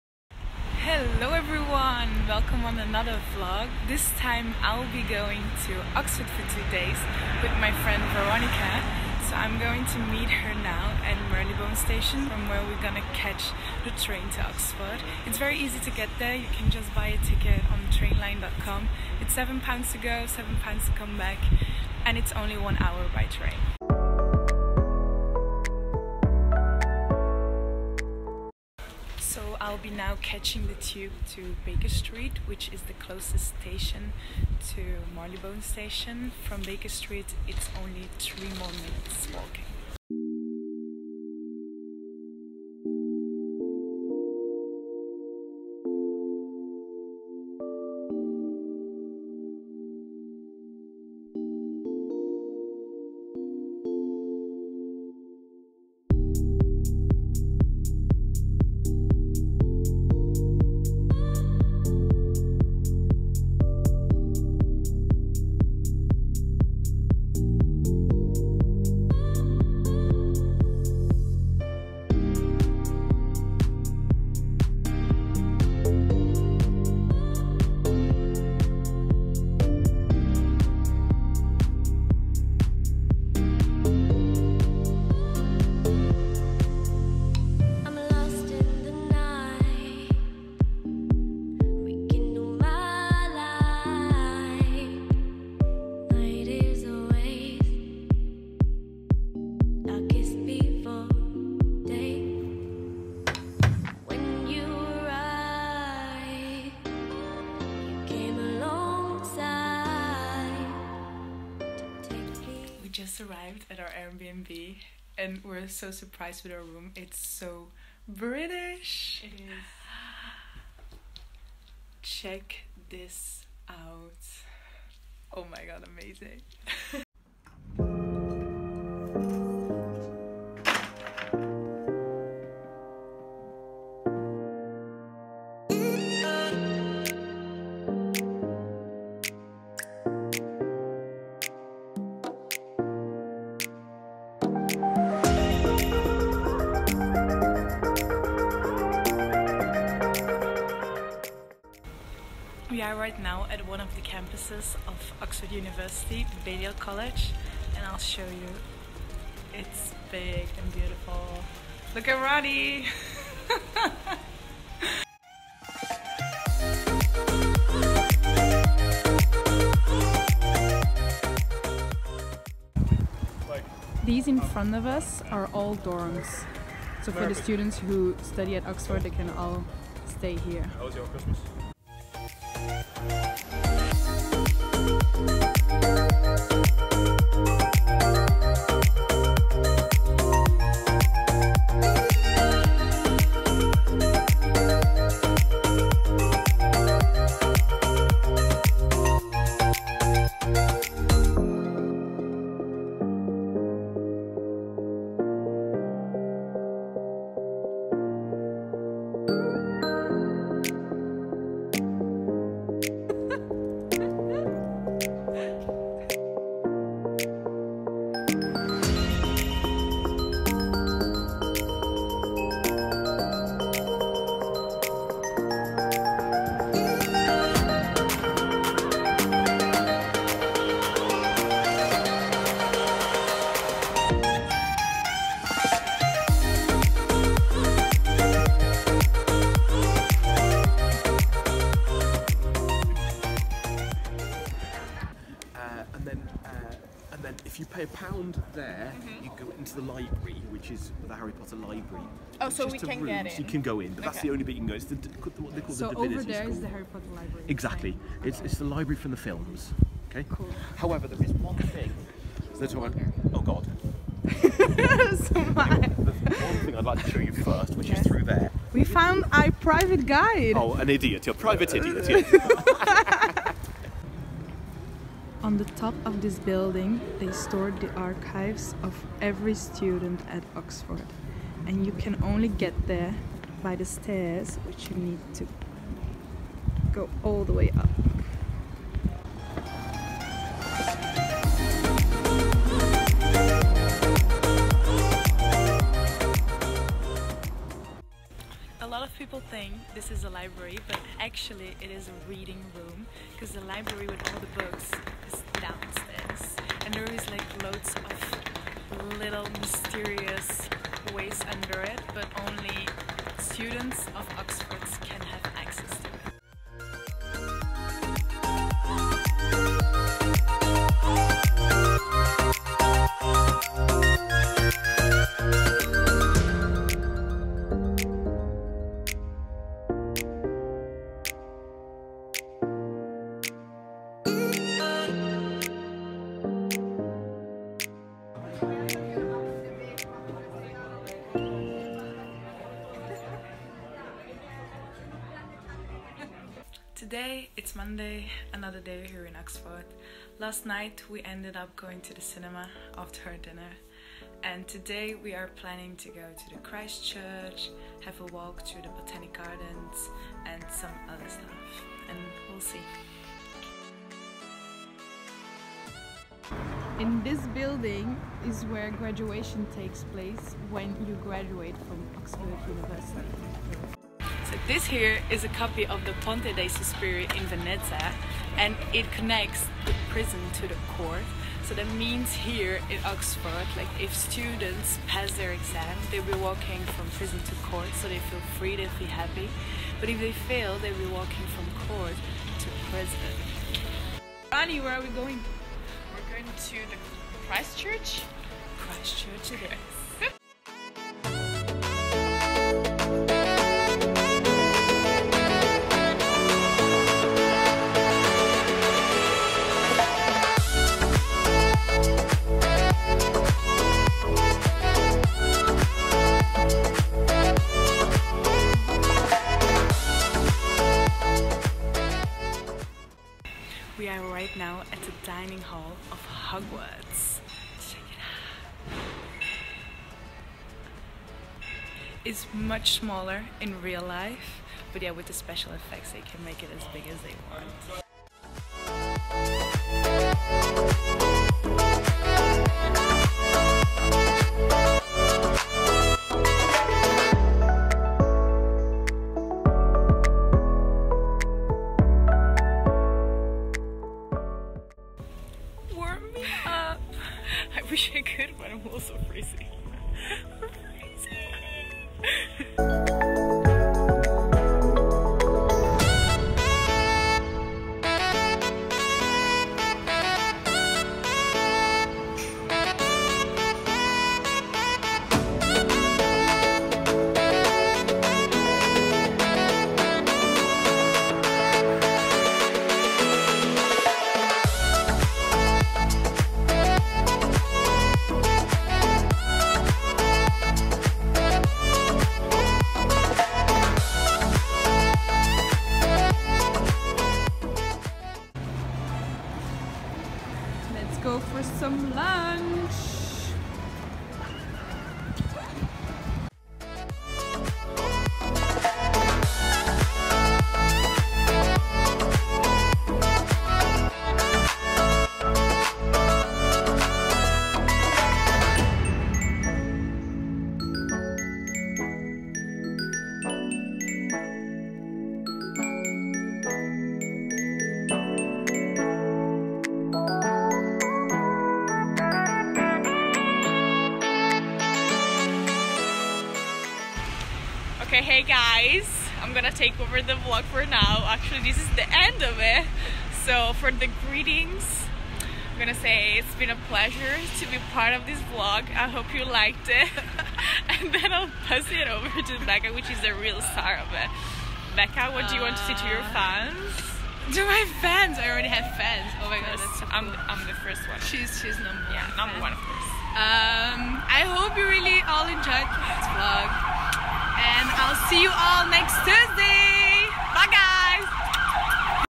TRIP TO OXFORD * VLOG 4
My friend and I took a trip to Oxford! Join us and discover the beauties of this city!